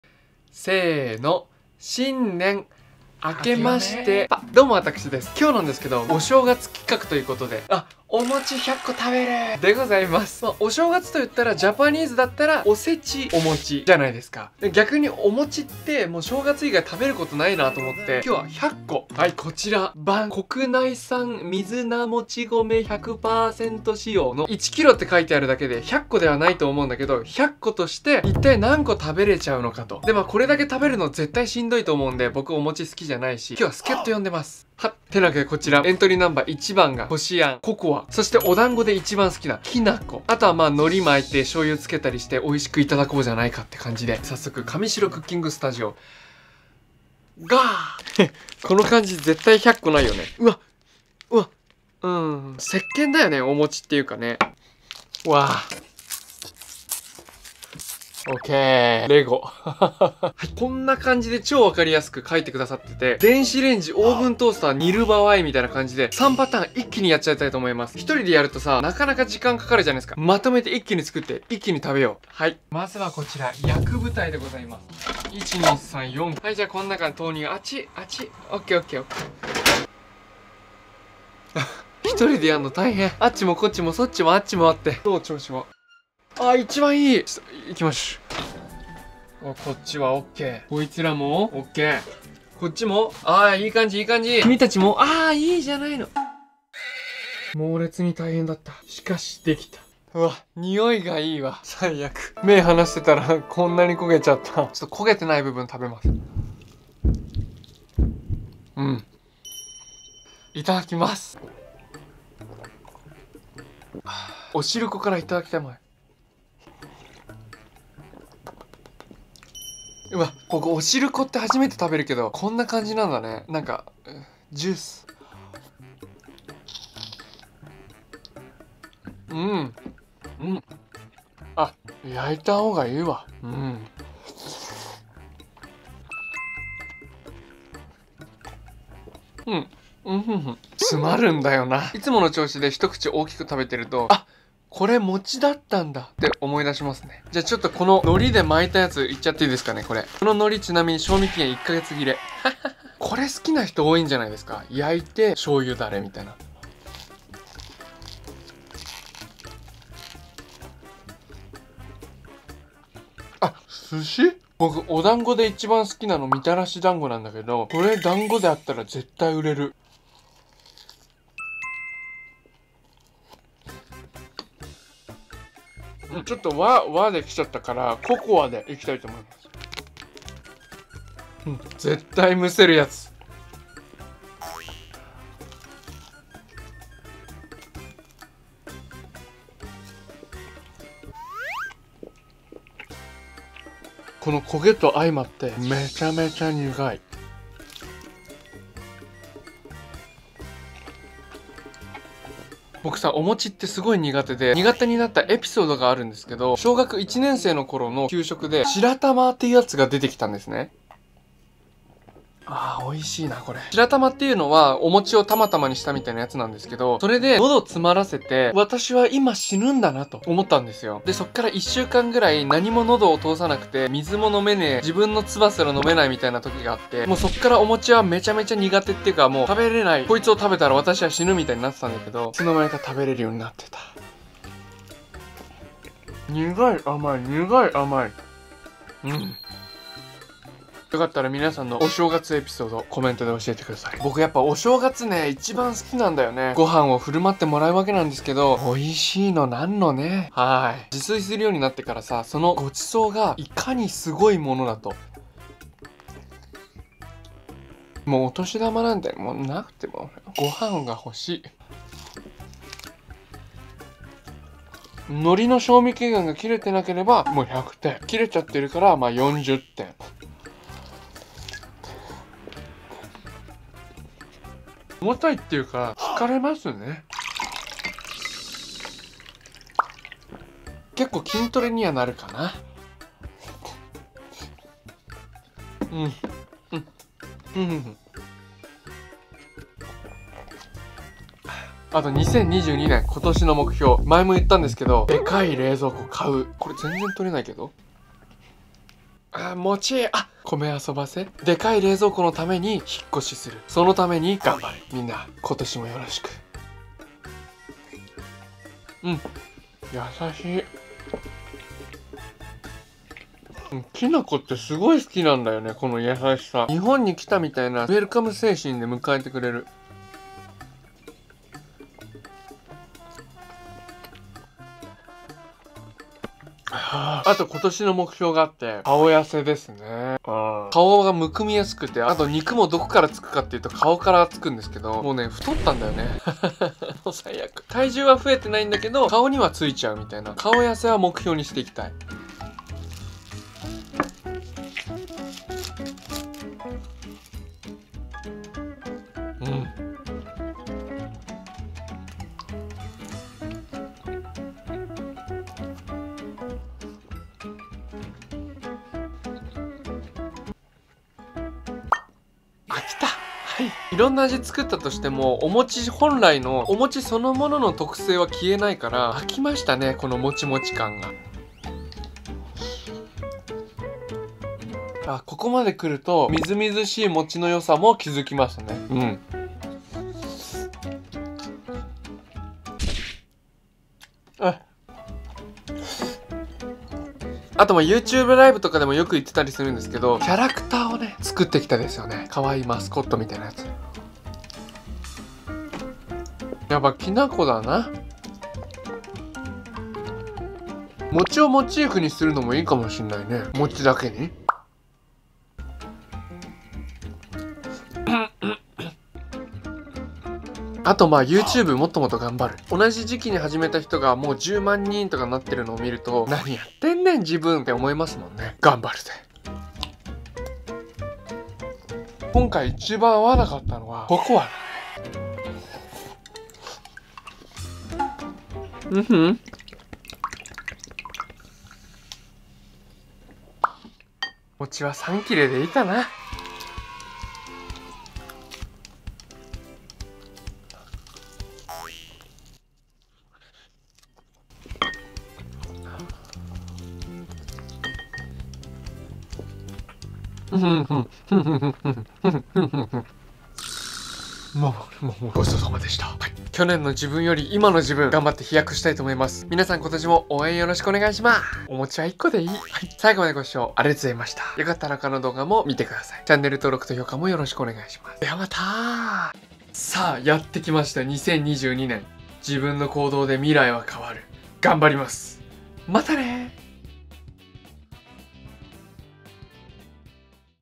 もちをアレンジして大食いしてみた【モッパン】
お正月っぽい企画したくてお餅1kg大食いしてみました。
今年の元旦はおせちの素晴らしさに気づけたので幸せです。

2022年の抱負は学ぶ心です。
和食中心の生活が続いたのでスイーツバイキングとか
食べ放題が恋しくなってきました。

グルメ旅も食べ歩きもしたいし忙しい一年なりそうです。
餅アレンジレシピよければ教えてください。
今年は料理も磨いていきたいです(去年も言ってた)

年間行事はクリスマスよりもバレンタインよりもお正月が好きなので
来年が楽しみです。

コメントいただけると飛び跳ねます。

ではまたっ(=ﾟωﾟ)ﾉ

BitStarアカデミー カミシロ担当者宛